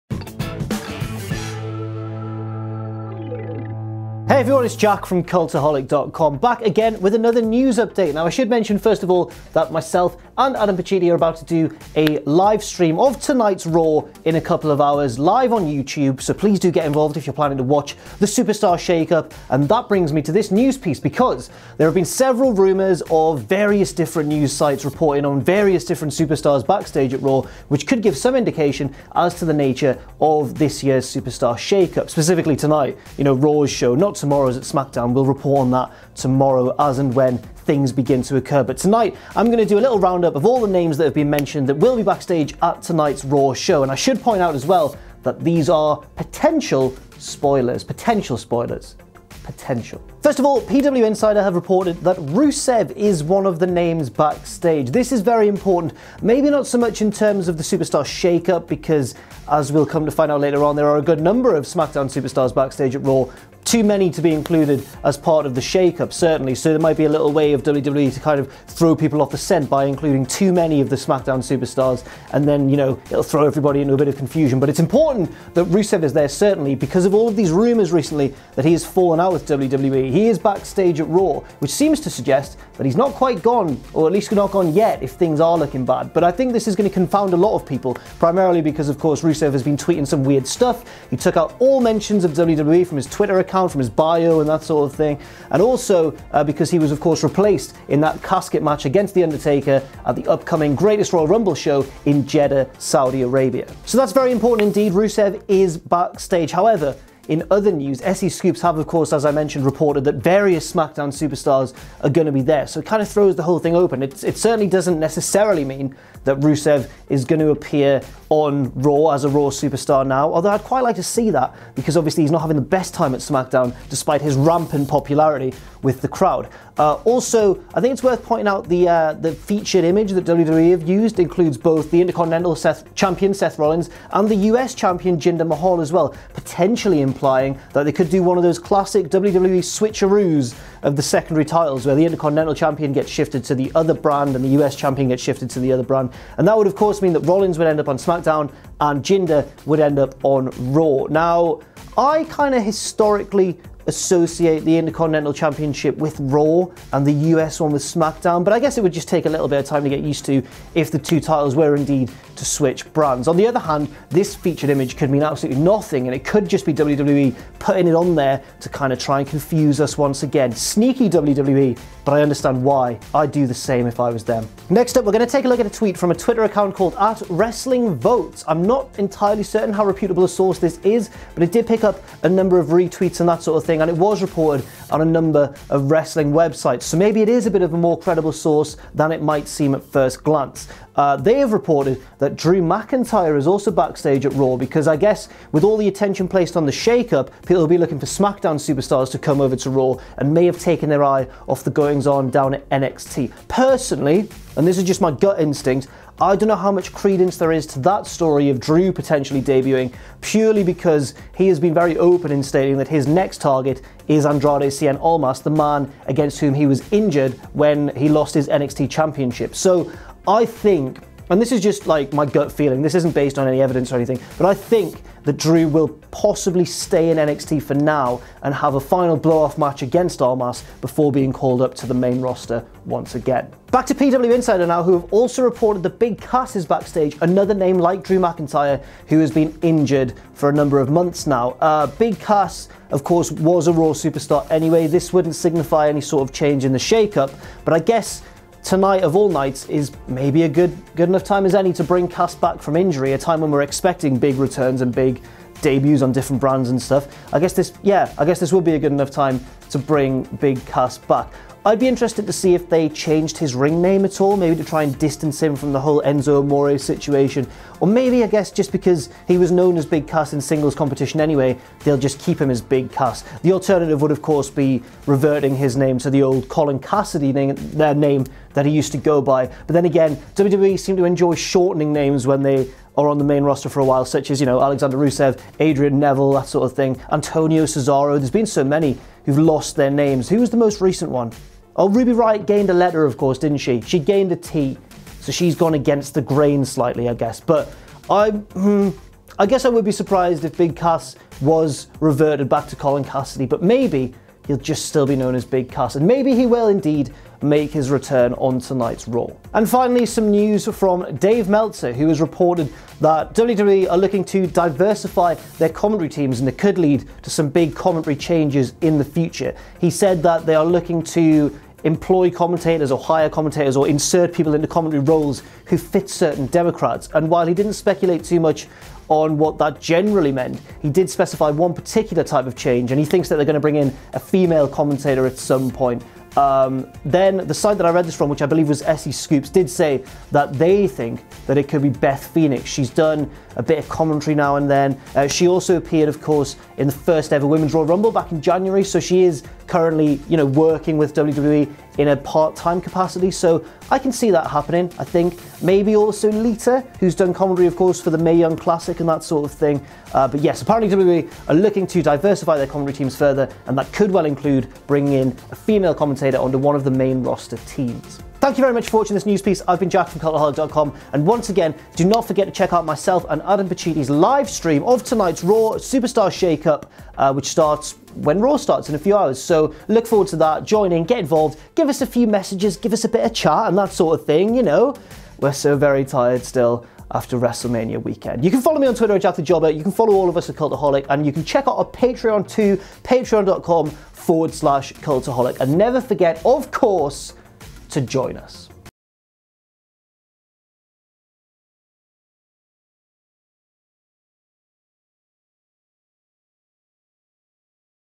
Potential Spoilers For Tonight's WWE Raw Superstar Shake-Up: Huge Names Backstage
We take a look at all the names reportedly backstage at tonight's Raw ahead of the Superstar Shake-Up, including various SmackDown Superstars, and potential NXT call-ups.